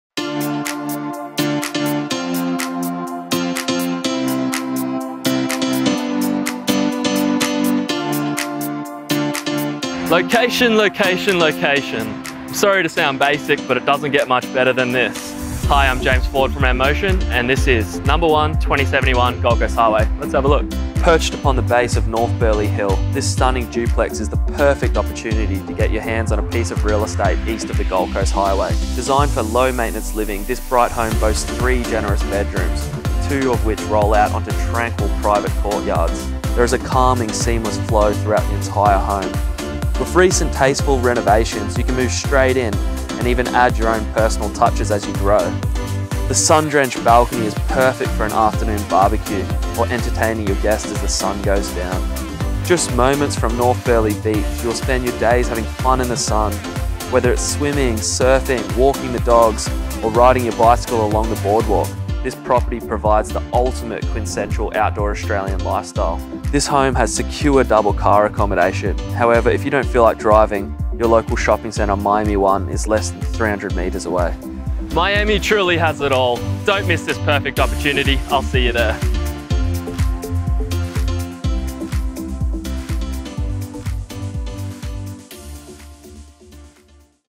For Sale By Auction (1/2071 Gold Coast Highway) ( M-MOTION
Perched upon the base of North Burleigh Hill,
this stunning duplex is the perfect opportunity
to get your hands on a piece of real estate east
of the Gold Coast Highway.

Designed for low maintenance living, this
bright home boasts 3 generous bedrooms, 2 of
which roll out onto tranquil private courtyards,
together with a calming, seamless flow
throughout the entire home. With recent
tasteful renovations you can move straight in.
The sun-drenched balcony is perfect for an
afternoon barbeque or entertaining your
guests as the sun goes down.

Just moments from North Burleigh Beach, you
will spend your days having fun in the sun.
Whether it's swimming, surfing, walking the
dogs or riding your bicycle along the
boardwalk, this property provides the ultimate
quintessential outdoor Australian lifestyle.

This home has secure double car
accommodation, however if you don't feel like
driving, your local shopping centre, 'Miami
One' is less than 300m away.

This property has it all. Make it yours today!